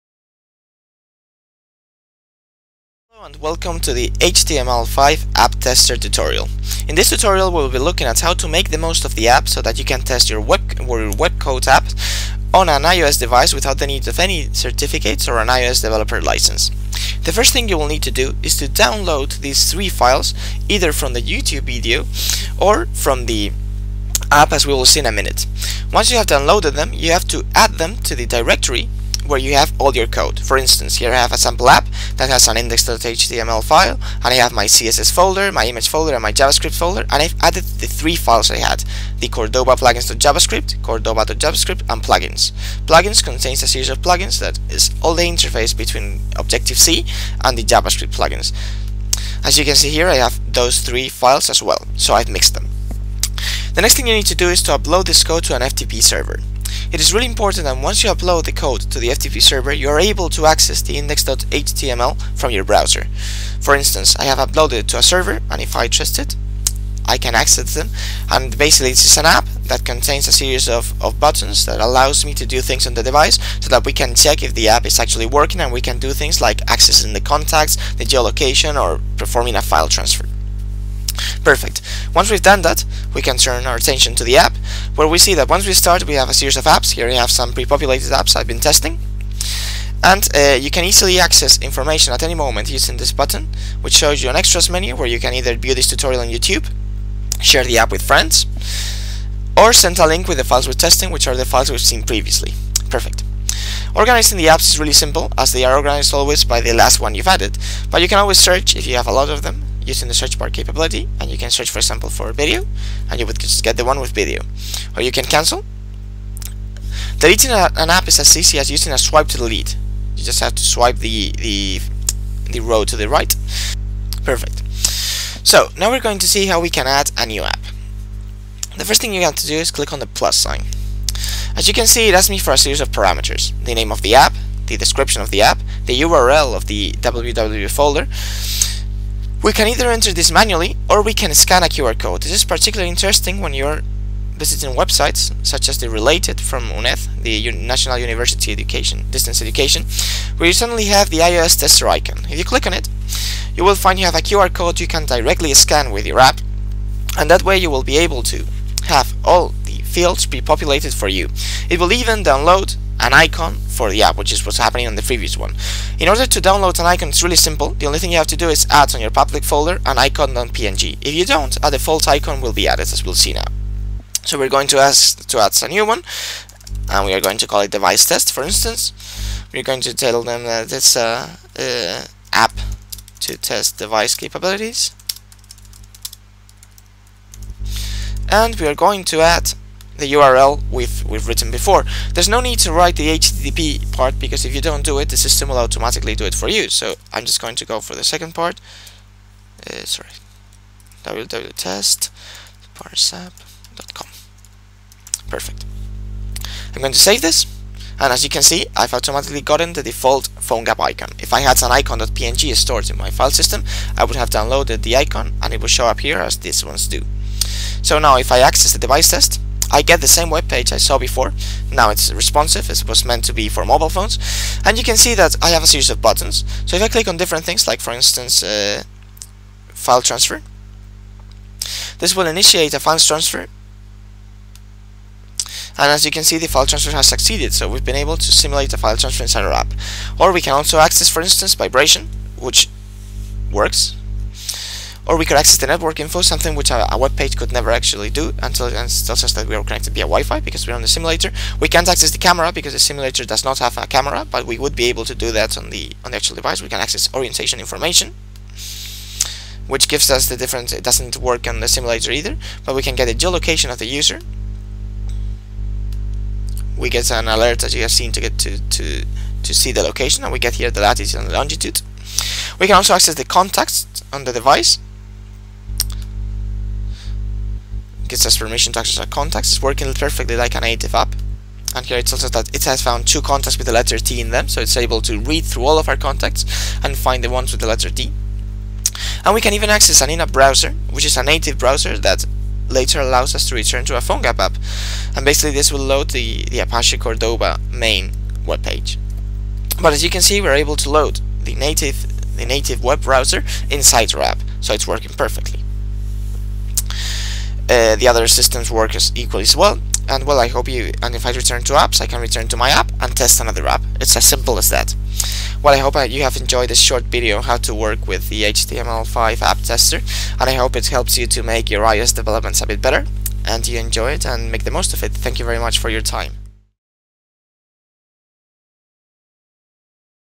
Tutorial HTMLAppTester - English
A brief tutorial on how to use HTML5AppTester for iOS.